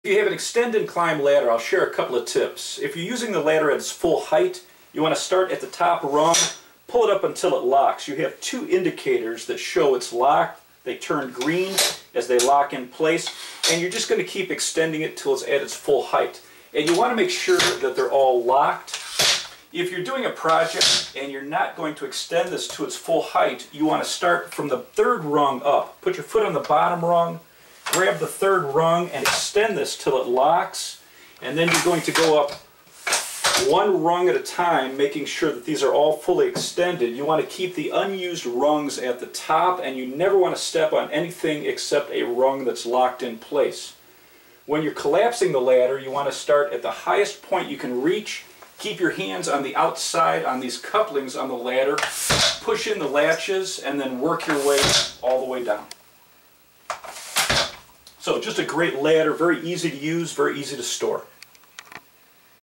Xtend and Climb Ladder Tips
I share some tips for Xtend & Ladders.

STEP 1:  EXTENDING THE LADDER
1.  Release the strap that secures ladder in its "storage configuration".
2.
a) Place ladder on firm level surface.
b) Place your foot on the bottom rung to hold ladder in place.
c) With both hands, pull up on the couplings of the 3rd rung from the bottom.
d) Make sure that each rung is pulled up to its full extension and locks before attempting to extend the next rung. The green dot will be visible when the lock is engaged. Both locks must be engaged on the each rung.
e) Extend desired number of rungs to achieve required height.
f) Do not stand or step on any rungs with 3 feet of the top of the ladder.
g) DO NOT climb higher than any rung that is not fully open. Keep retracted rungs at the  top of the ladder.
TIP:  To extend the ladder to its fullest height, pull up the top rung first, raising the rung to its full extension. Make sure it locks into place. Continue with each remaining rung. Again, make sure each rung locks into place before extending the next rung.

STEP 2: CLOSING THE LADDER
Great care should be used when closing the ladder. Do not place any part of the body including hands, fingers or thumb on or between rungs or couplings above the closing rung.
TIP:  Please remember that just as the ladder extends and locks in one foot increments, it also closes in foot increments. Each rung closes by simultaneously pushing the latches on the previous, lower rung inward towards one another.
Starting at the highest extended rung, please close each rung by securely placing your thumbs on the latches below the rung that you are closing. Please make sure that the rest of your hand is below the plastic coupling of the rung.
Step by Step:
1.  Grasp the ladder by the couplings below the rung that you are activating the latches.
2.  Place thumbs on latches that you wish to disengage.
3.  Place the rest of your hand below the coupling.
4.  If you place the rest of your hand above the coupling- your hand could be pinched between the rung!
5.  Simultaneously push latches inward (towards each other) until the previous rung is lowered.
6.  Repeat this operation for each rung.
SAFETY FEATURE:  If the next lower rung is fully extended, the closing rung will stop before hitting the rung that you are holding.  This closing rung will nestle completely when the next lower rung is disengaged.